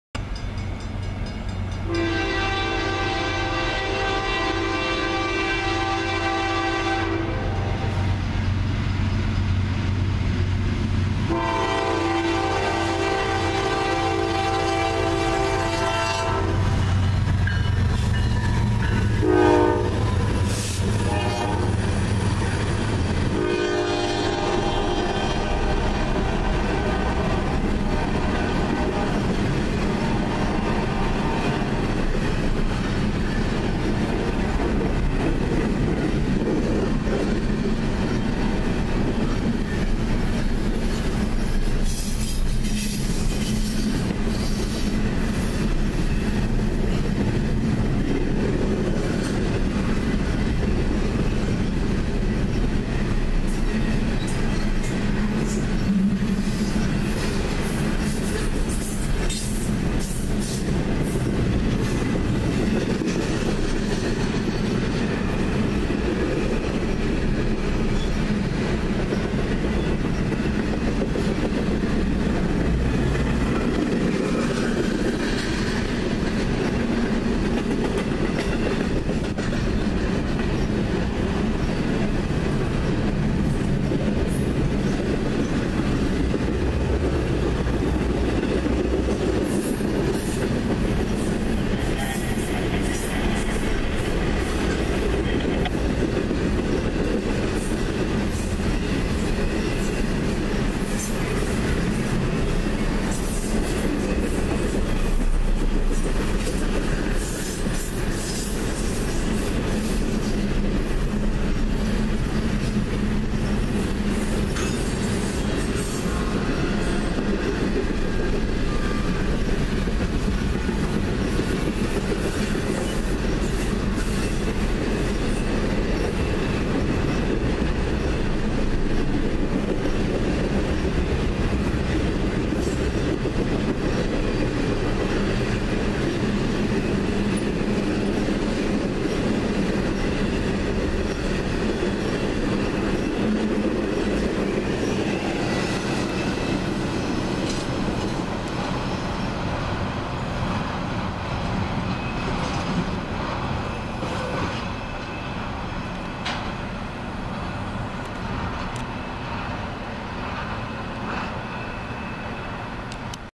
Eastbound intermodal with NS 7565 as the leader
What a great day for trains today.  Just got off my pm run with my job and afterwards I came down to my usual spot and started off with this intermodal going east with a DC GEVO leader and an ACe trailing, but I got some better ones too, so please stay tuned in and don't touch that dial!  Enjoy this while the others are still pending.